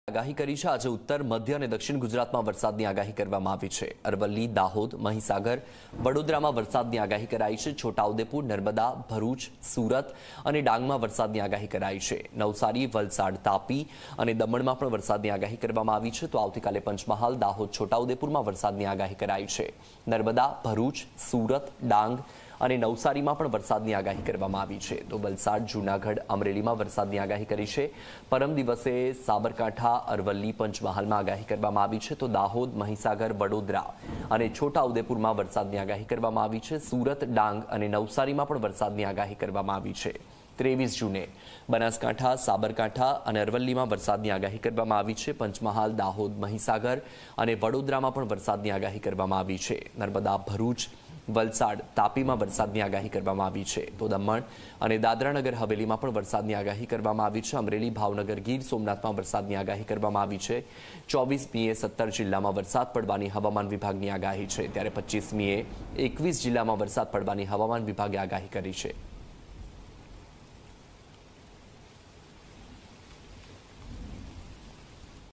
Weather Forecast: Rainfall , gusty winds predicted in THESE parts of Gujarat for next five days
ગુજરાતમાં 5 દિવસ વરસાદની હવામાનની આગાહી  --

આજે ઉત્તર, મધ્ય અને દક્ષિણ ગુજરાતમાં વરસાદની આગાહી --

અરવલ્લી, દાહોદ, મહીસાગર, વડોદરામાં વરસાદની આગાહી --

છોટાઉદેપુર, નર્મદા, ભરૂચ, સુરત અને ડાંગમાં પણ આગાહી --
નવસારી, વલસાડ, તાપી અને દમણમાં પણ વરસાદની આગાહી --
આવતી કાલે પંચમહાલ દાહોદ, છોટાઉદેપુરમાં વરસાદની આગાહી  --

નર્મદા, ભરૂચ, સુરત, ડાંગ અને નવસારીમાં પણ વરસાદની આગાહી  --

વલસાડ, જૂનાગઢ, અમરેલીમાં વરસાદની હવામાનની આગાહી --

પરમ દિવસે સાબરકાંઠા, અરવલ્લી, પંચમહાલમાં આગાહી --
દાહોદ, મહીસાગર, વડોદરા અને છોટાઉદેપુરમાં આગાહી --

સુરત, ડાંગ અને નવસારીમાં પણ વરસાદની આગાહી  --

23 જૂને બનાસકાંઠા, સાબરકાંઠા અને અરવલ્લીમાં આગાહી  --

પંચમહાલ, દાહોદ, મહીસાગર અને વડોદરામાં આગાહી  --

નર્મદા, ભરૂચ, વલસાડ, તાપીમાં વરસાદની આગાહી  --

દમણ અને દાદરાનગર હવેલીમાં વરસાદની આગાહી  --

અમરેલી, ભાવનગર, ગીર સોમનાથમાં વરસાદની આગાહી --

24મીએ 17 જિલ્લામાં વરસાદ પડવાની હવામાનની આગાહી  --

25મીએ 21 જિલ્લામાં વરસાદ પડવાની હવામાનની આગાહી --